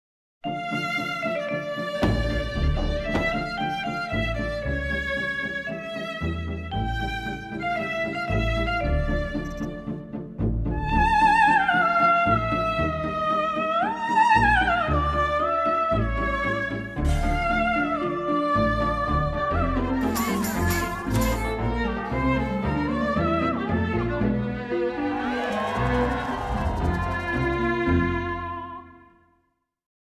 SRFF 2019 - Official Festival Trailer - 30 sec version
This is the SR Socially Relevant Film Festival New York sixth edition official trailer 30 second version, for the festival that takes place March 15-21, 2019.